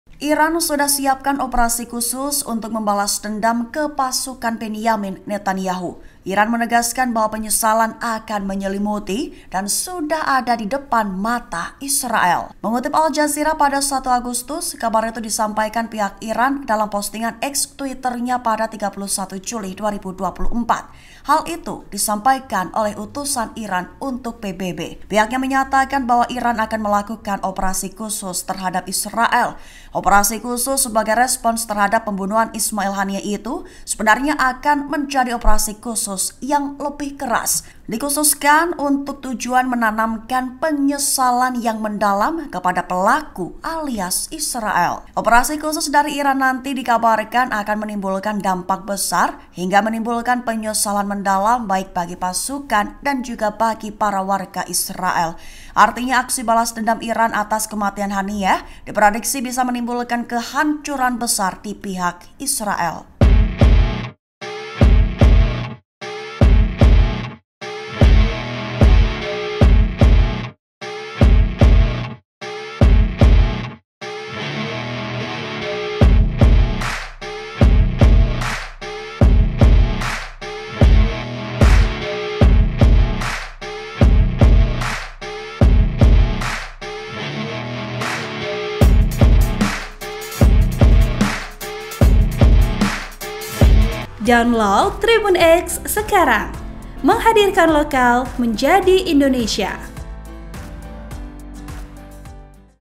Iran Sudah Siapkan Operasi Khusus Balas Dendam ke Pasukan Netanyahu, Penyesalan di Depan Mata Israel
TRIBUN-VIDEO.COM - Iran sudah siapkan operasi khusus untuk balas dendam ke pasukan Benjamin Netanyahu.

Iran menegaskan bahwa penyesalan akan menyelimuti dan sudah ada di depan mata Israel.

Mengutip Al Jazeera pada (1/8), kabar itu disampaikan pihak Iran dalam postingan X (Twitter) nya pada (31/7/2024).

Hal itu disampaikan utusan Iran untuk PBB.

Pihaknya menyatakan bahwa Iran akan melakukan operasi khusus terhadap Israel.

Operasi khusus sebagai respons terhadap pembunuhan Ismail Haniyeh itu sebenarnya akan menjadi operasi khusus yang lebih keras.

Dikhususkan untuk tujuan 'menanamkan' penyesalan yang mendalam kepada pelaku alias Israel.

"Iran akan melakukan operasi khusus terhadap Israel, respons terhadap pembunuhan tersebut sebenarnya akan menjadi operasi khusus yang lebih keras yang bertujuan untuk menanamkan penyesalan yang mendalam pada pelaku" - Utusan Iran untuk PBB.

Operasi khusus dari Iran nanti dikabarkan akan menimbulkan dampak besar hingga menimbulkan penyesalan mendalam, baik bagi pasukan dan juga  bagi para warga Israel.

Artinya, aksi balas dendam Iran atas kematian Haniyeh diprediksi bisa menimbulkan kehancuran besar di pihak Israel.

(Tribun-Video.com)

Artikel ini telah tayang di Tribunnews.com dengan judul Iran akan Balas Dendam usai Terbunuhnya Ismail Haniyeh, Siapkan Operasi Khusus Bikin Israel Menyesal,
Penulis: garudea prabawati
Editor: Pravitri Retno W

Program: Tribun Video Update
Host: Yessy Arisanti Wienata
Editor Video: Dandi Bahtiar
Uploader: bagus gema praditiya sukirman